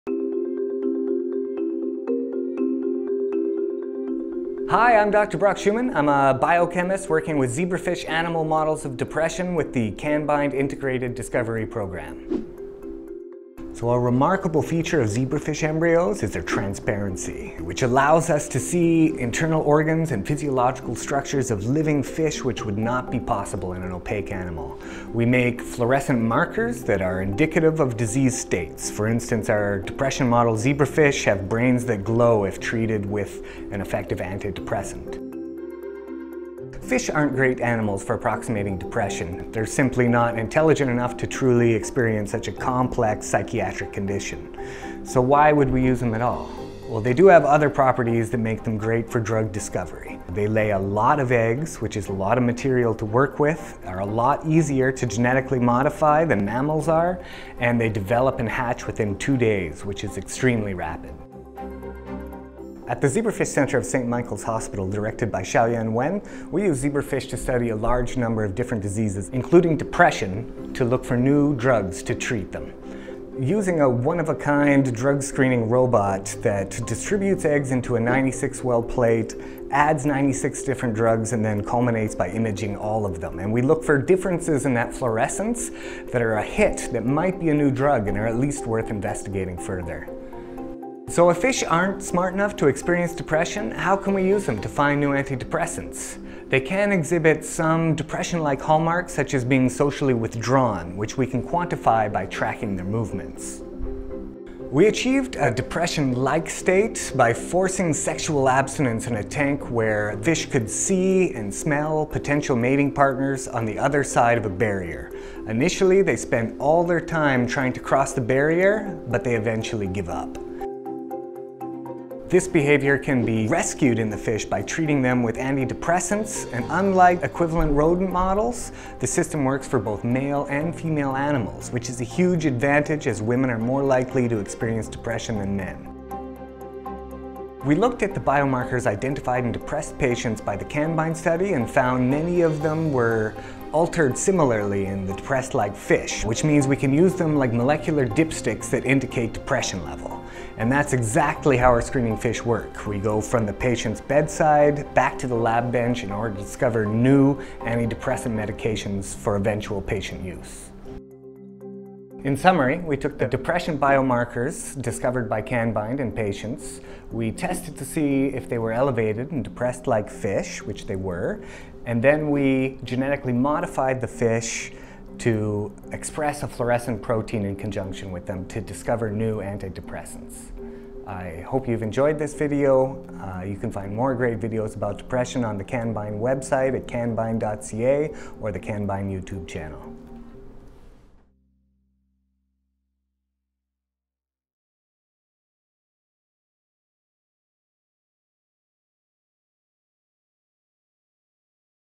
Can Glowing Fish Help With Depression Research?
Dr. Brock Schuman of The CAN-BIND Program tells us how glowing, see-through zebrafish can help with depression research.